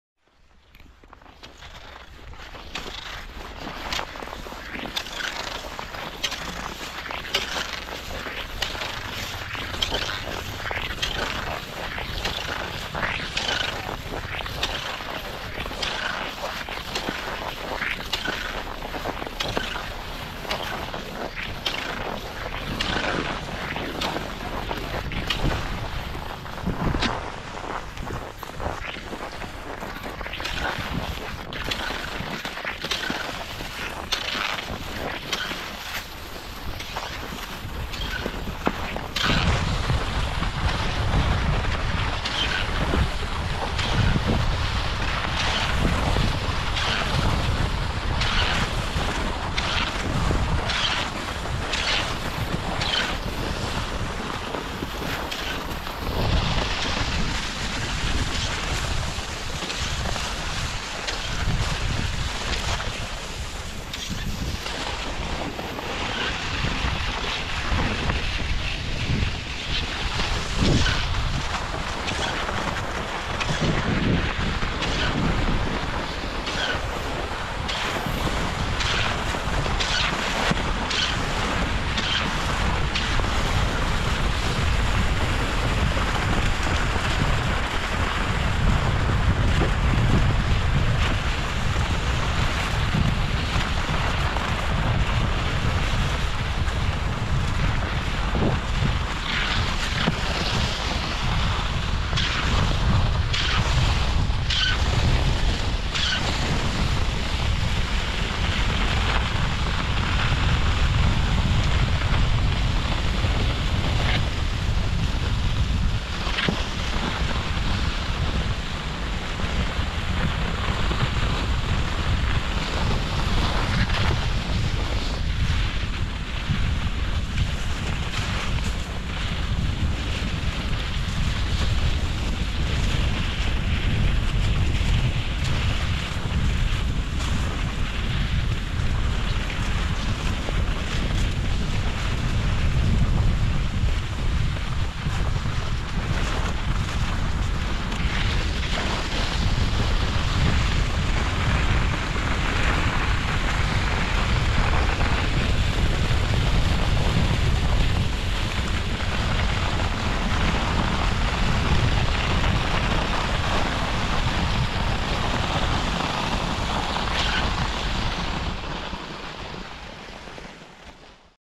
Saariselkä Kaunispää. Pitkä laskettelu lapissa tuntureilla. GPS nopeustiedot
Saariselkä Kaunispää. Pitkä laskettelu lapissa tuntureilla. GPS nopeustiedot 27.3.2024.